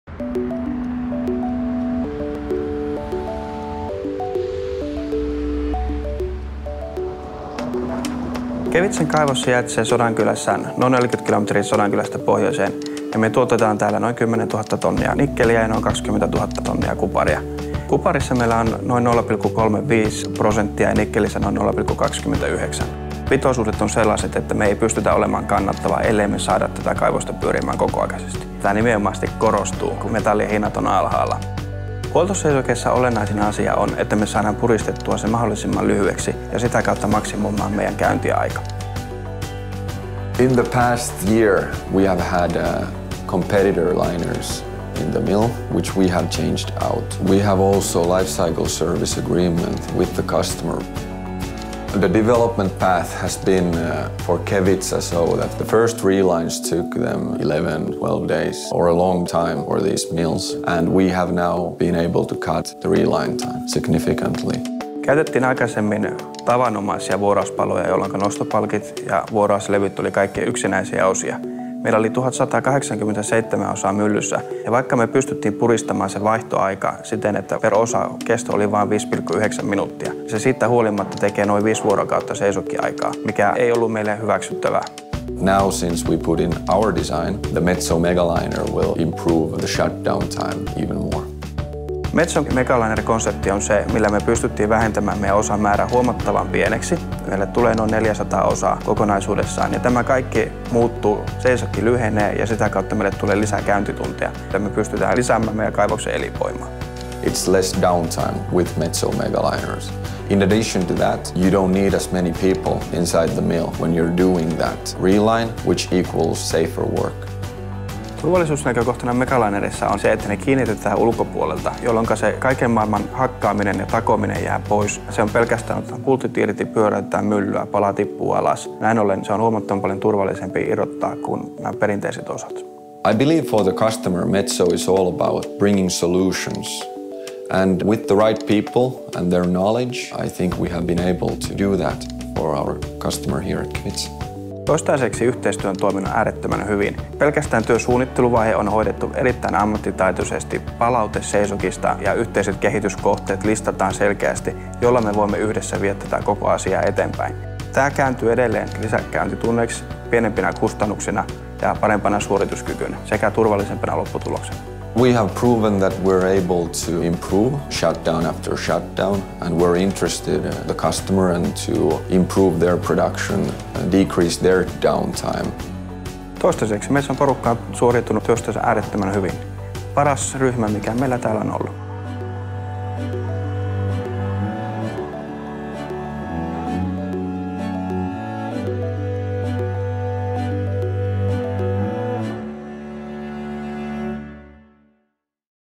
Speeding up mill relines at FQM Kevitsa
At FQM's Kevitsa copper-nickel mine in northern Finland, each minute of uptime counts. With the help of Metso, the mine has cut the duration of shutdowns and increased the availability of the comminution process. A recent switch to Metso’s Megaliner™ mill linings and crusher wear parts will offer Kevitsa an even greater advantage in their quest for maximum uptime.